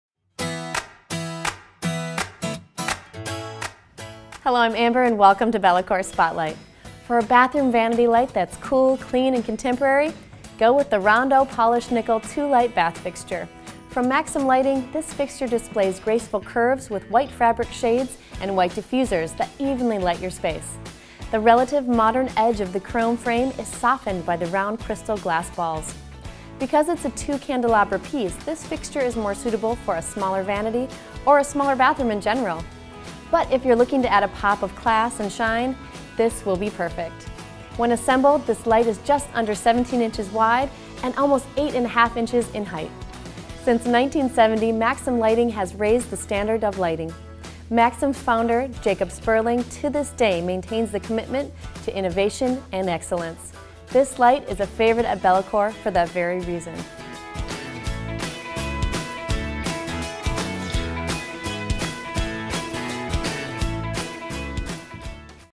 Maxim Lighting Rondo Two-Light Bath Fixture - Bellacor
For a bathroom vanity light that’s cool, clean and contemporary, go with the Rondo Polished Nickel Two-Light Bath Fixture.

Finish: Polished Nickel
Dimensions: 16 3/4"W x 8 1/4"H x 8"Ext
Backplate: 6 1/2"W x 4 49/100"H
Height from Center of Outlet: 6 13/100"
Brand SKU: 12762WTPN